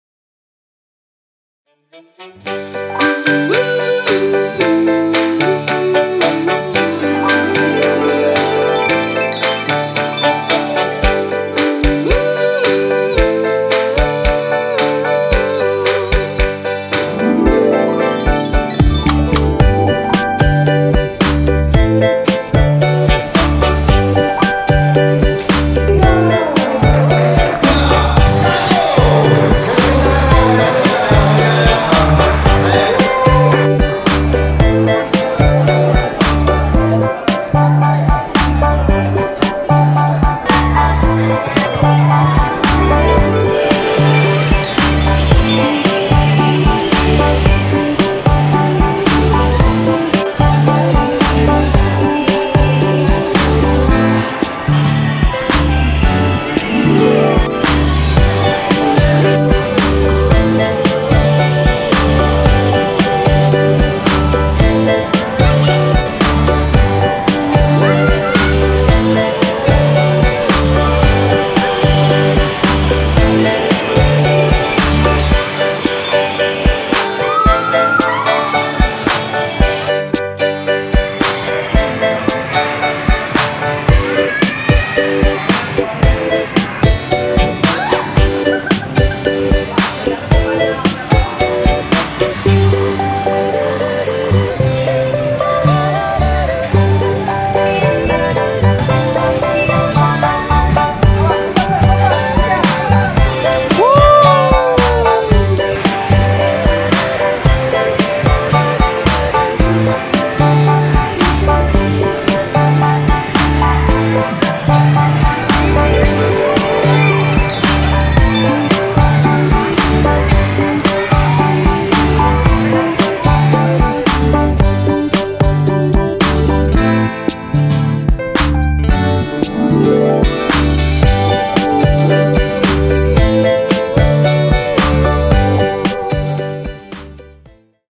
Indonesia Ramayana Indoor Playground Project
In April 2024, 1850m2 space theme indoor playground was completed installation in Indonesia. Here is the normal day of our customers' indoor playground. Do you want to invest in commercial indoor playground equipment?